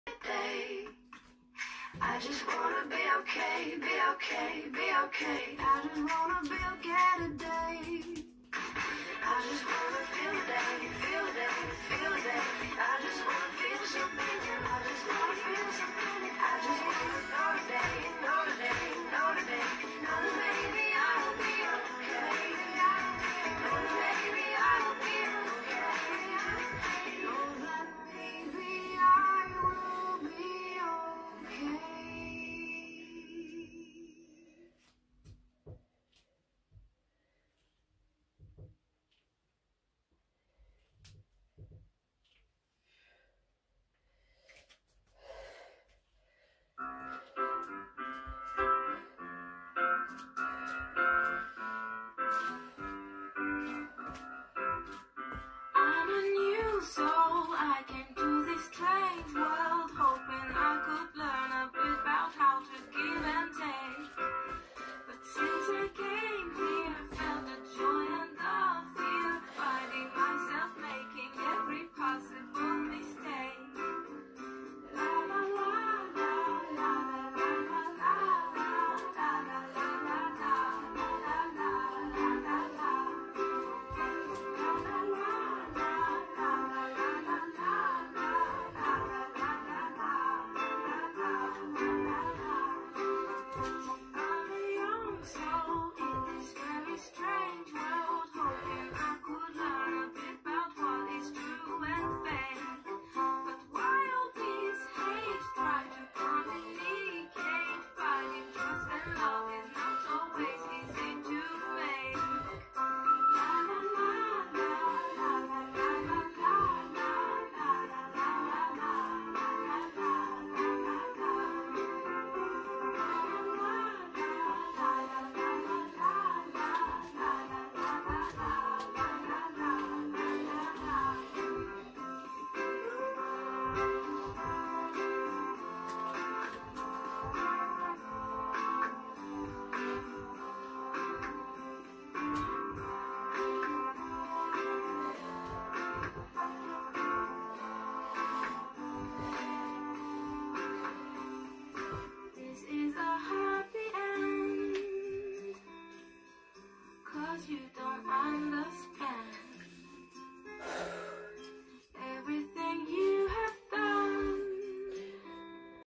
Quarantine workout 🔥💪 homemade Trx
Today was special! I improvised a handmade Trx and smashed a good circuit which included 4 exercises! Each exercise performed for 20s,no rest, then repeat circuit 5-8 times AMAZING
No equipment?! No problem!